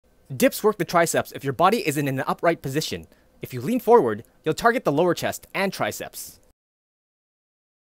DIPS: Muscles Worked (DO THIS!)
Dips work mainly to the triceps if you body is in an upright position.  If you lean forward, you will shift some of the focus to the lower chest.